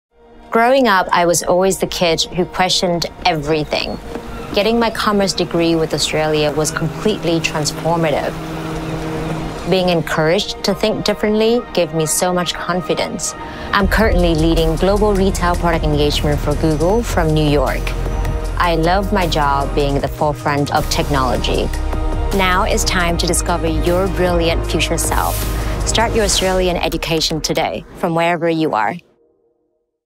Chi Vu - Discover your brilliant future self (30 seconds)
Chi Vu’s career has taken her from Vietnam to Singapore to the US, working for some of the world’s largest multinational companies. Chi credits her Australian education with kickstarting her successful marketing career and giving her the confidence to take professional risks. It's time to discover your brilliant future self to find out how.